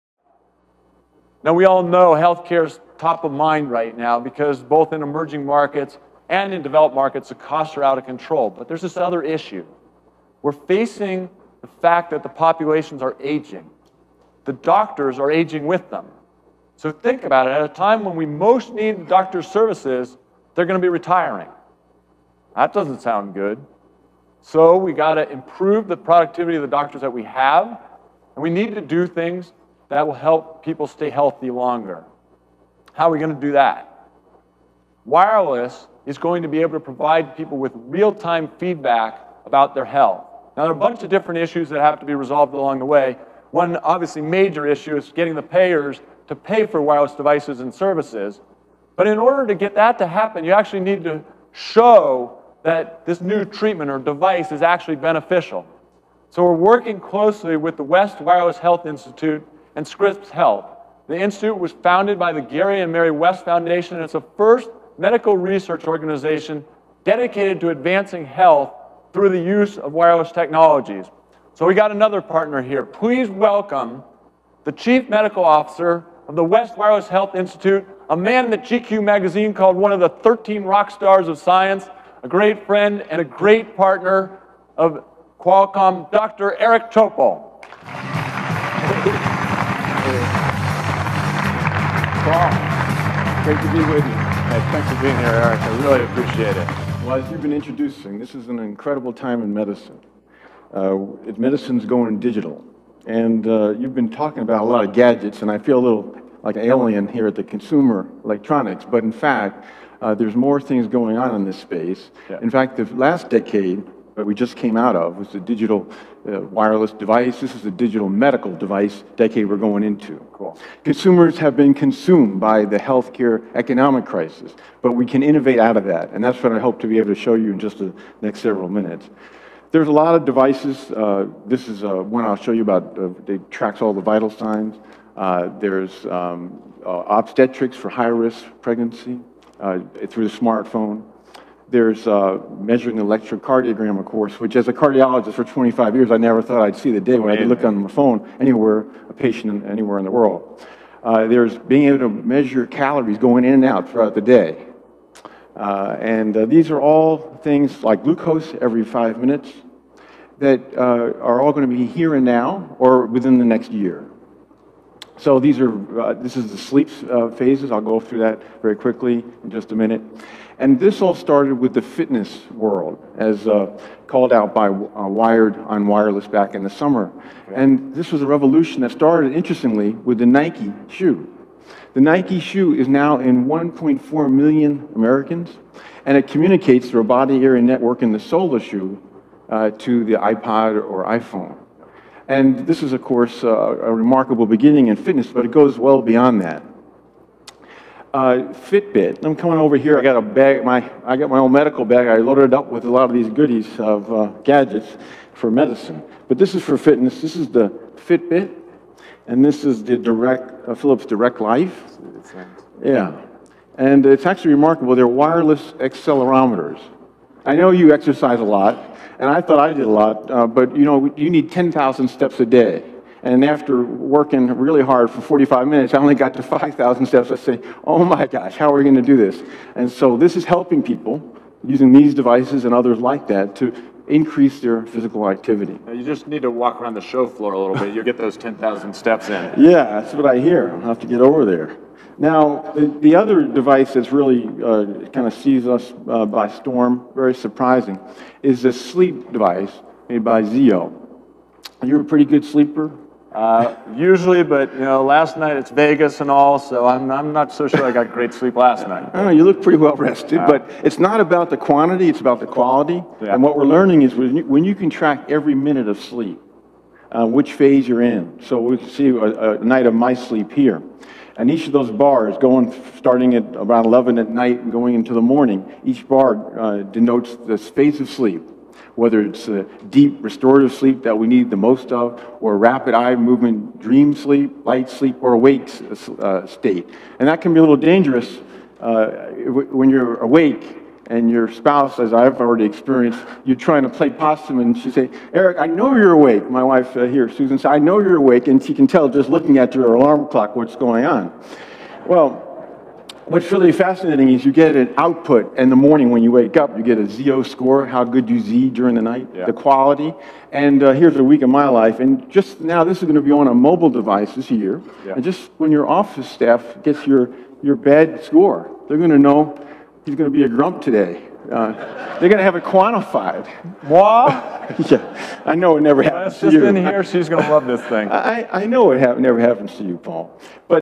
CES 2010 - Transforming Healthcare Part 1, feat. Dr. Topol of West Wireless Health Insititute
Wireless health hits the big stage at CES International 2010 - January 8, 2010
As noted in coverage by the Wall Street Journal, Dr. Eric Topol's live demonstration of digital medical devices like GE's handheld ultrasound Vscan was a real cause for optimism.  "Consumers have been concerned about the economic crisis" in medicine, Topol said. "But we can innovate out of that.". Watch Dr. Eric J. Topol highlight a variety of mhealth solutions, including the first live demo of GEs Vscan technology.  It's no wonder consumers will have a tremendous impact on transforming healthcare.